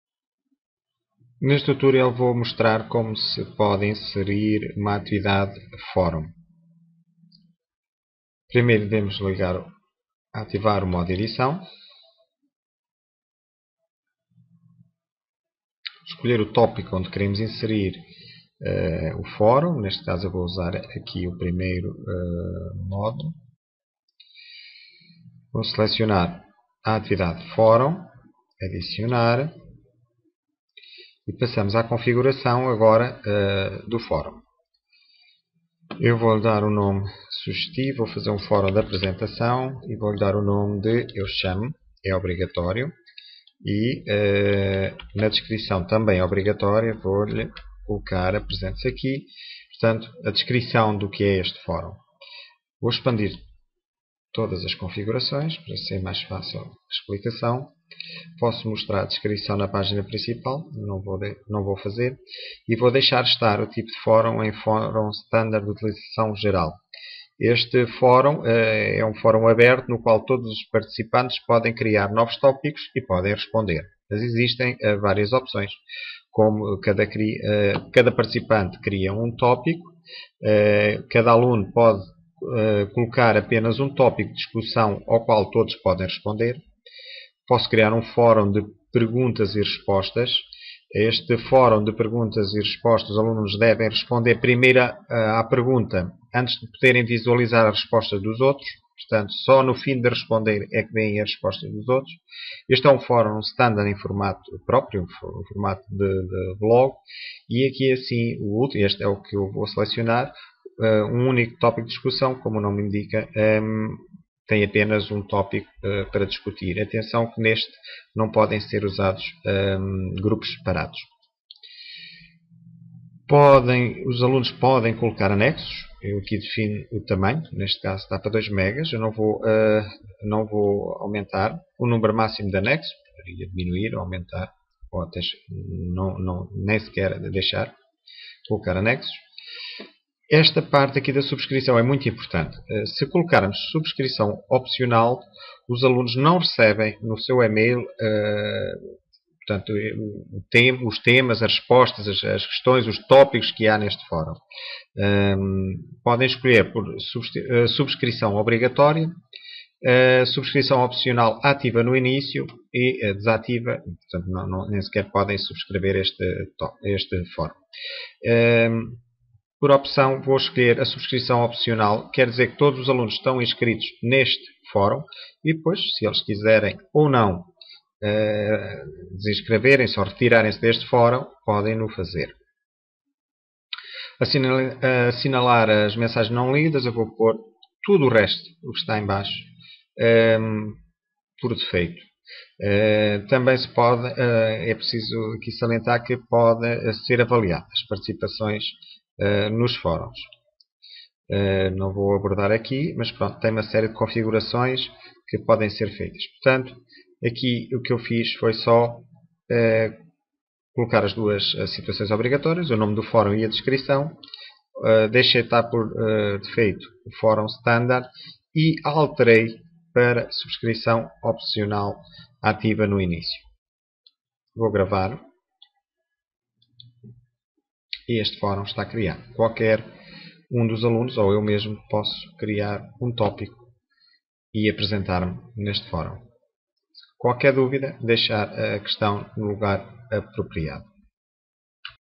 Tutorial - Atividade Fórum
Moodle - Inserir e configuração de uma atividade fórum.